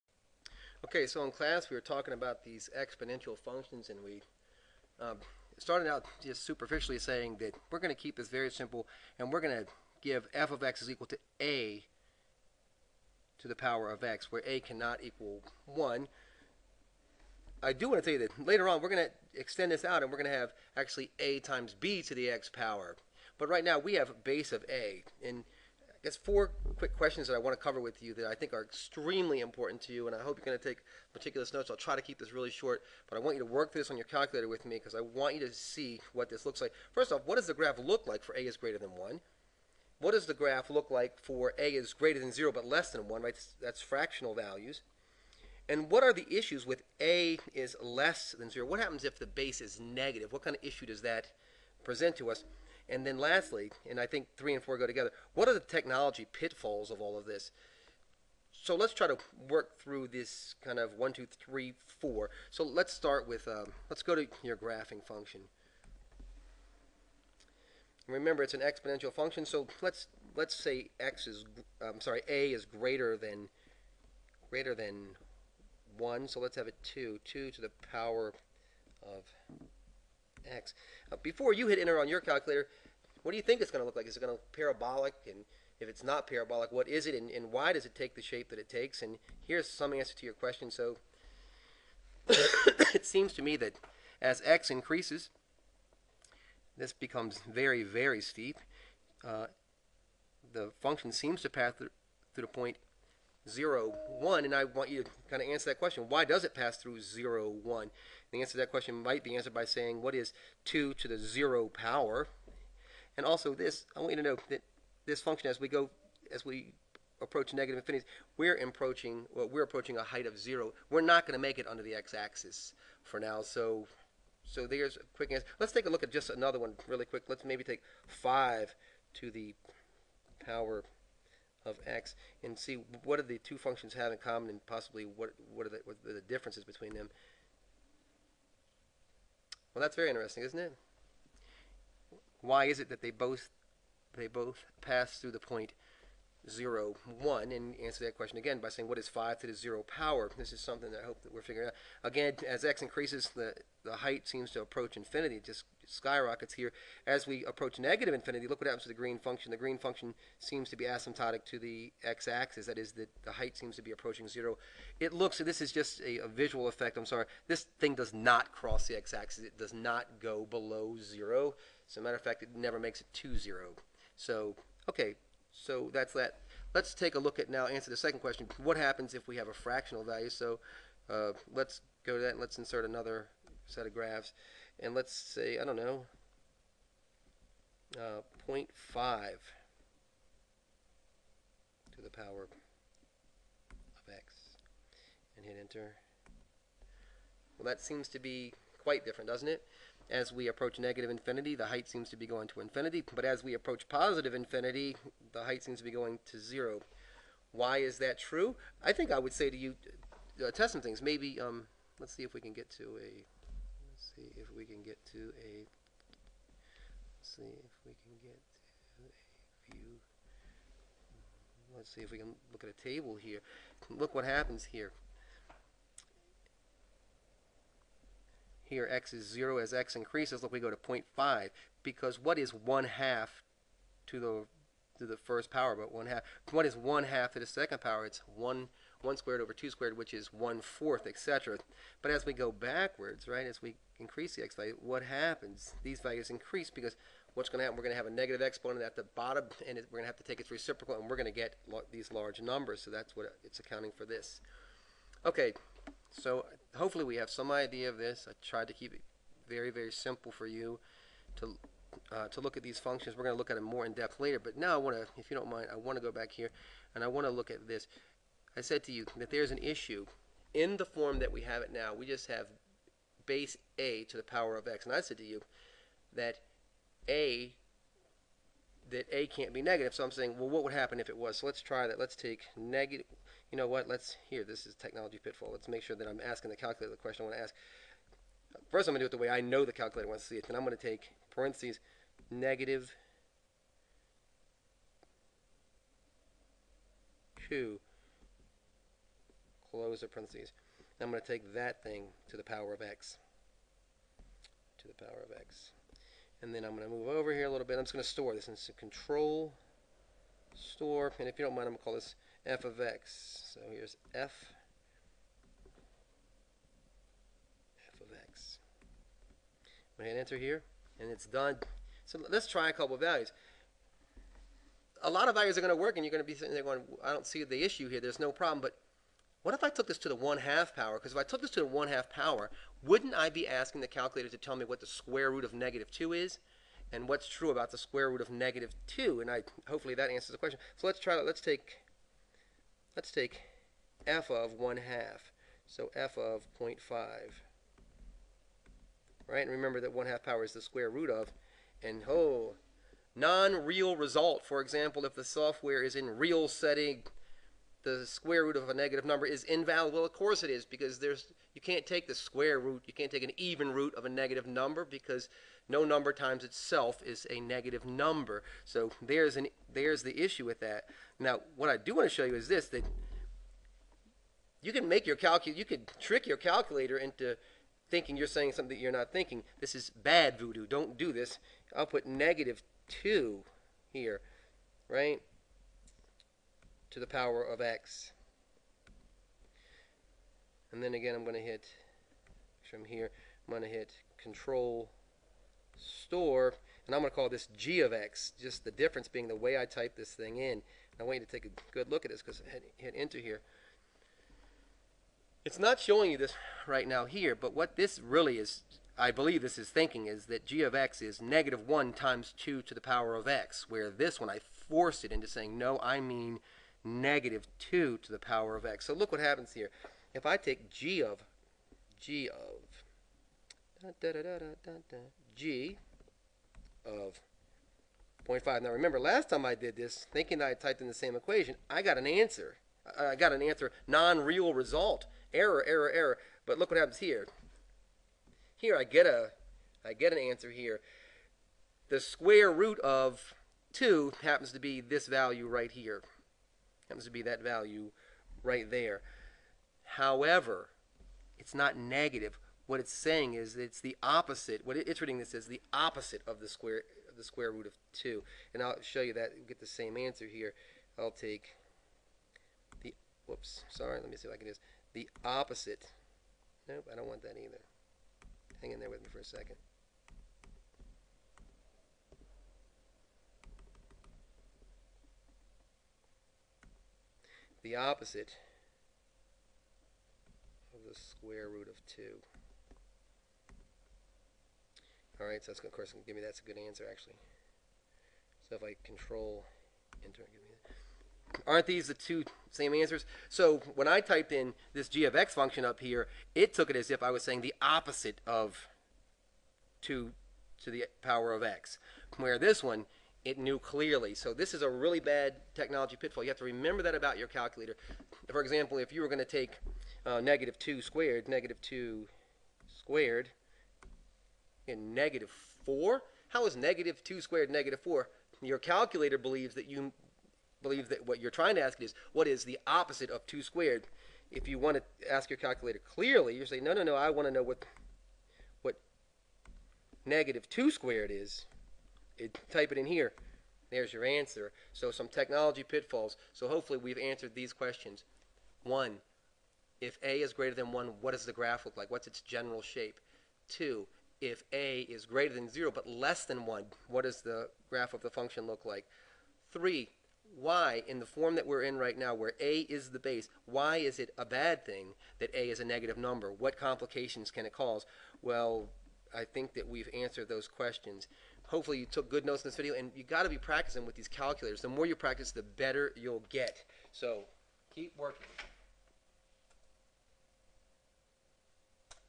Graphs of Exponential Functions (technology trap) SAT Prep
This is a very simple of visualizing the graph of an exponential function.  The video describes the possible values of the base and what general shapes can be expected.  The graph also demonstrates some very simple characteristics of exponential functions.  The graphs of demonstrated on the Ti-Nspire CAS calculator, technology pitfall and helpful calculator hints are provided.